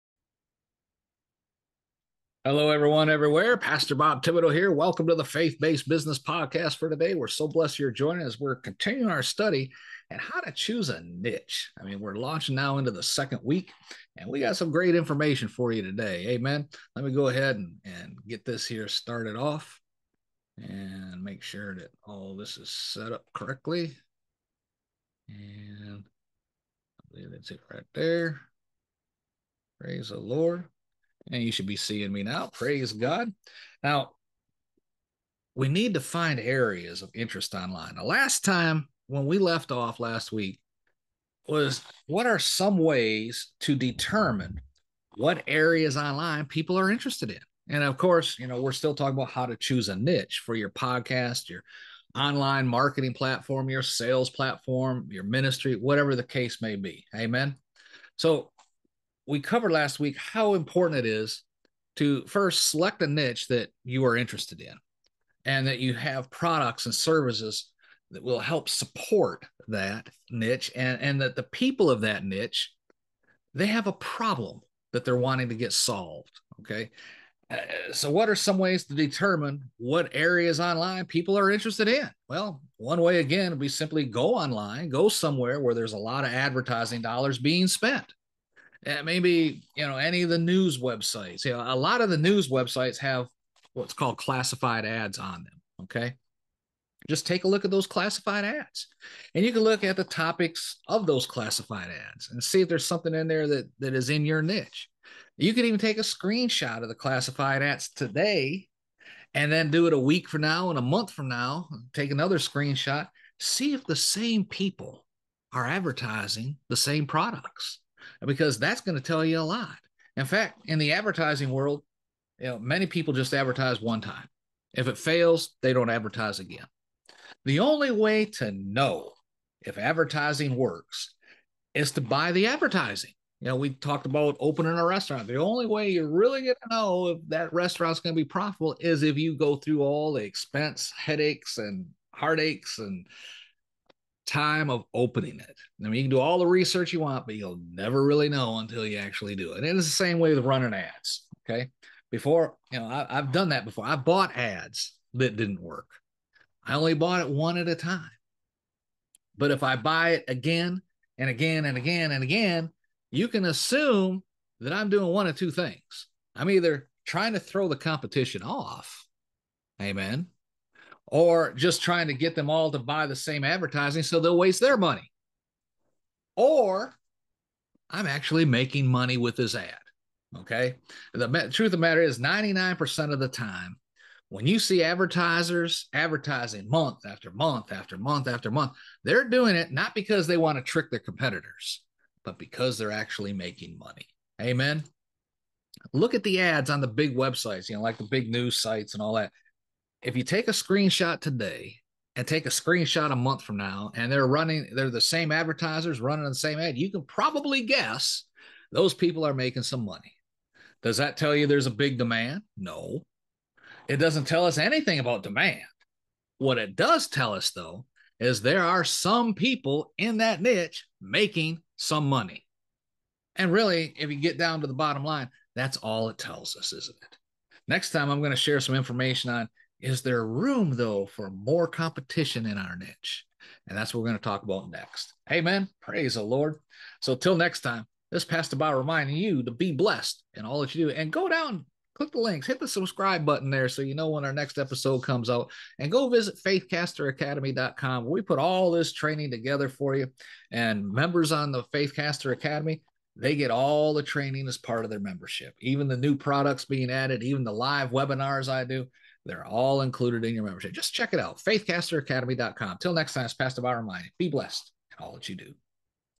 Finding God’s Path in Business: How to Discover What People Need Online
Are you seeking God’s guidance on what your business should offer? In this episode, we’ll explore how to find out what people are searching for online—by looking at trends, advertising, and where the demand really is. I’ll show you how to tap into what God may be leading you to offer, so you can build a business that not only serves others but also glorifies Him. Let’s discover together where the opportunities lie for your faith-based business!